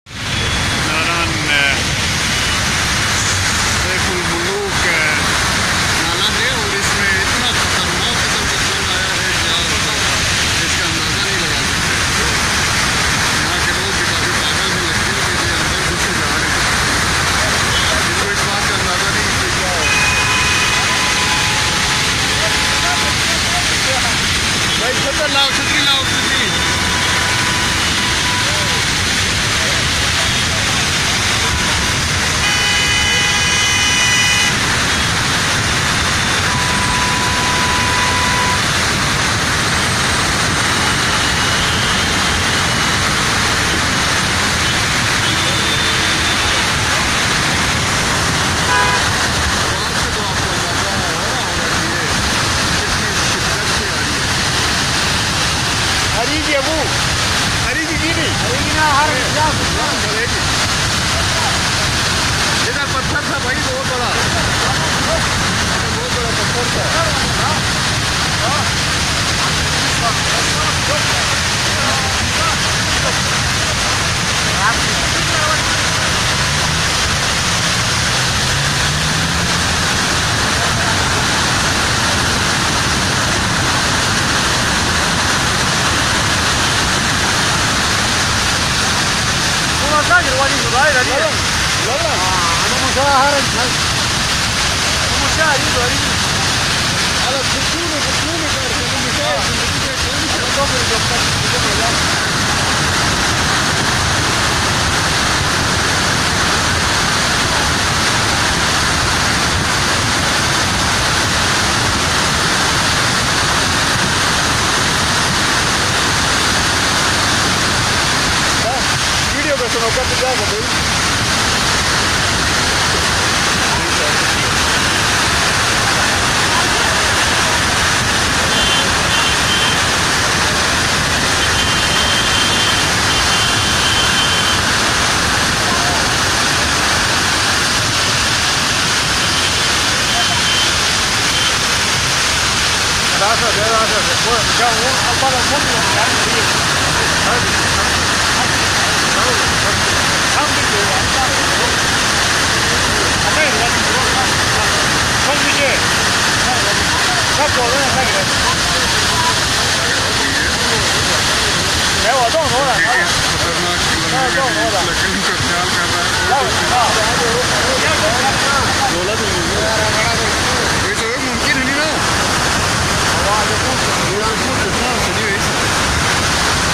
Flash flood in Saifulmalook channel
A sudden rain breaks the floods in channels and RiverNaran.